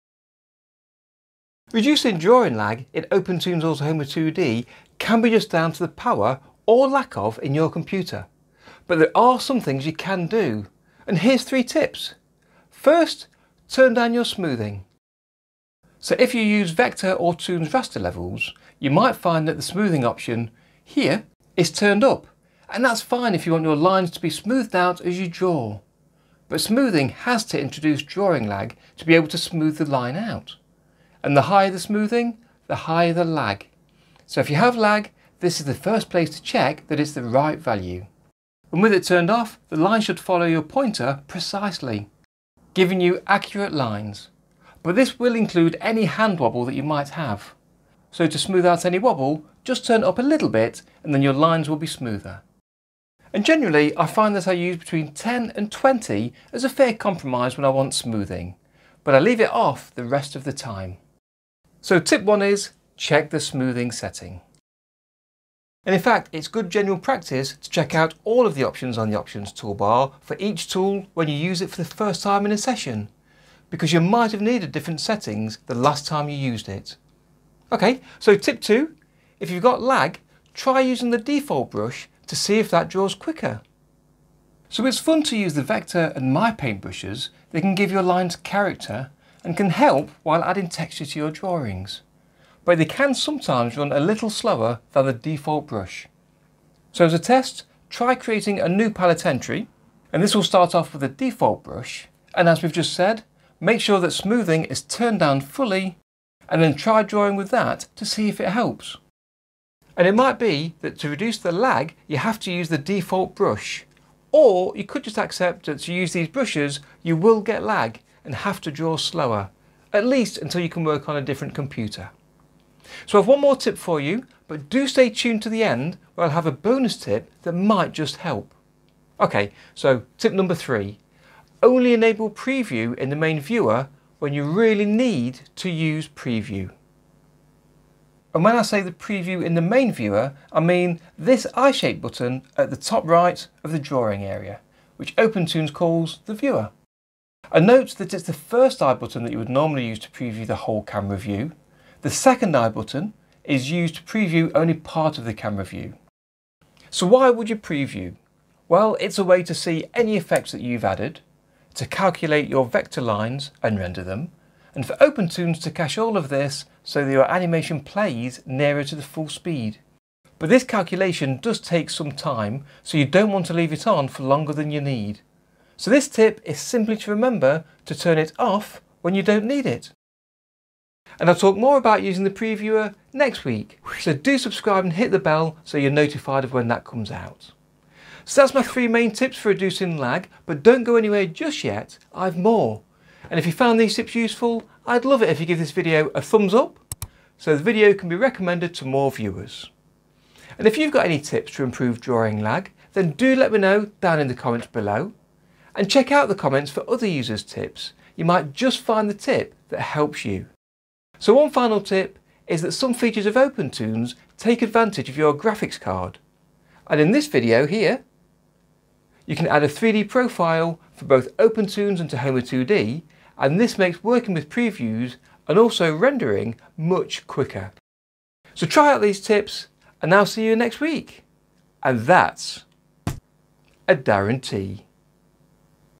My top 3 tips to reduce drawing lag in OpenToonz and Tahoma2D
Here's my top 3 tips to help reduce drawing lag.

Contents:
00:00 Tip1: Turn down your smoothing
01:17 Tip 2: Try the default brush
02:09 Tip 3: Only enable the viewer preview when you need to
03:07 Bonus tips

Extra tips:
- Add OpenToonz & Tahoma2D to your Windows High Performance graphics options.

If you liked what you see and want to help out, you treat me a coffee or a Darren Tea ;-)

Discord; for help setting up and using OpenToonz, chat with animators & developers and enter animation challenges. And my other socials: